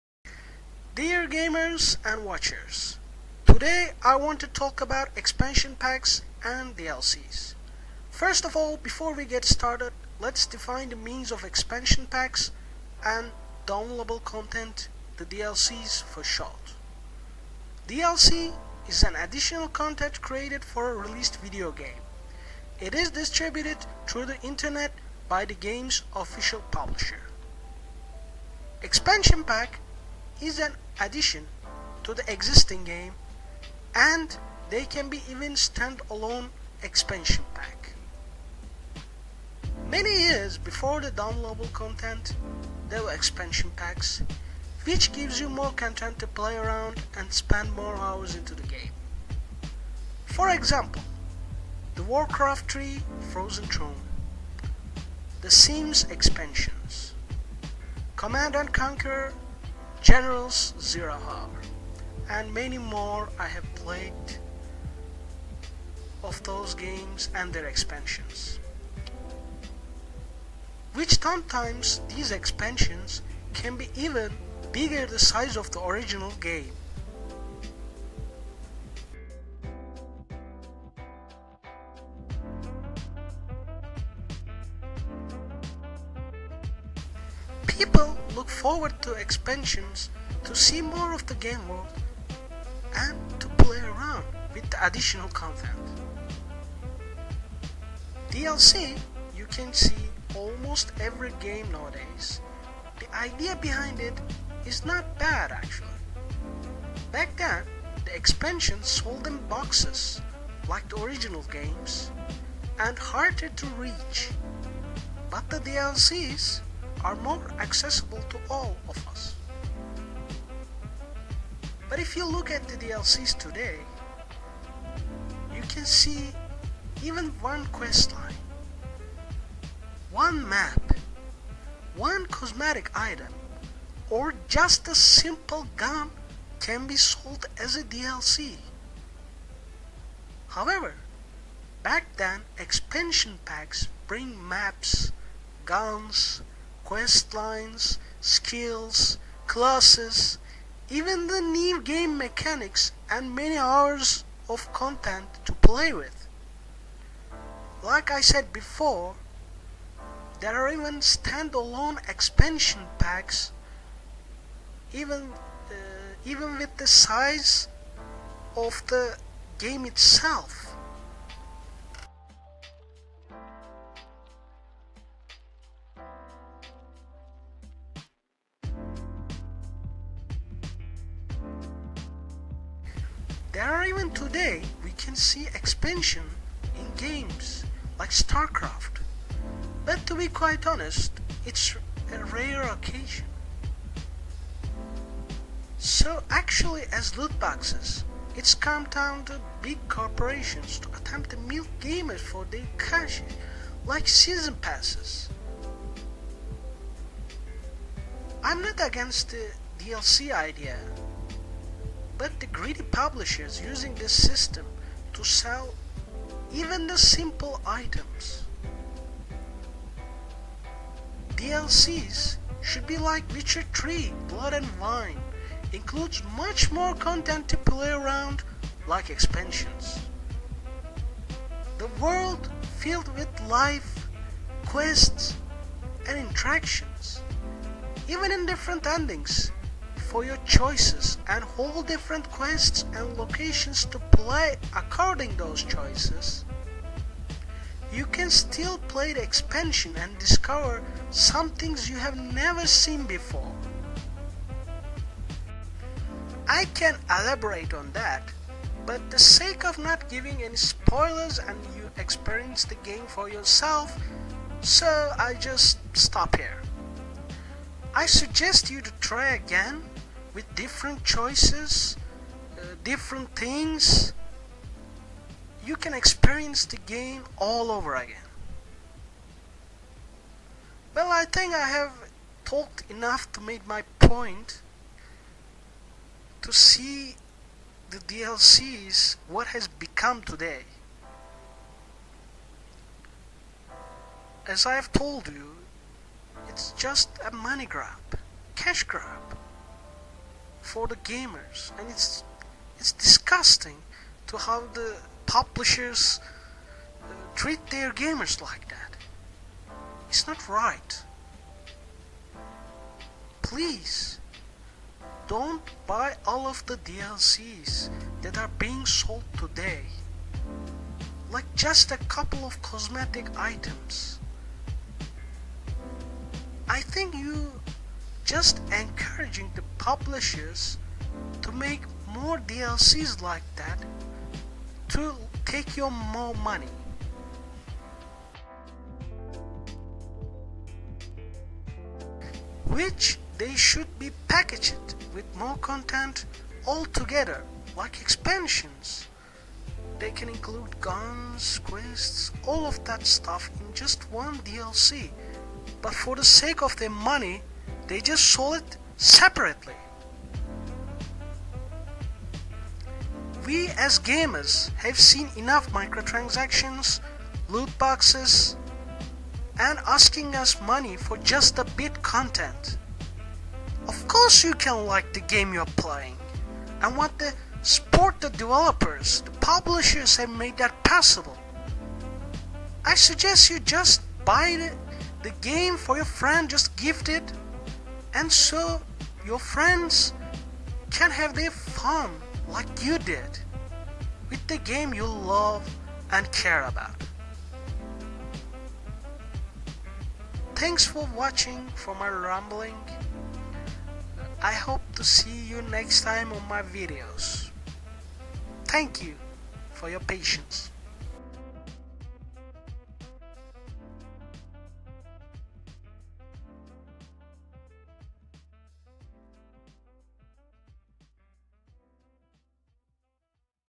Lets talk about DLCs and Expensions Packs
i want to talk about DLCs and Expensions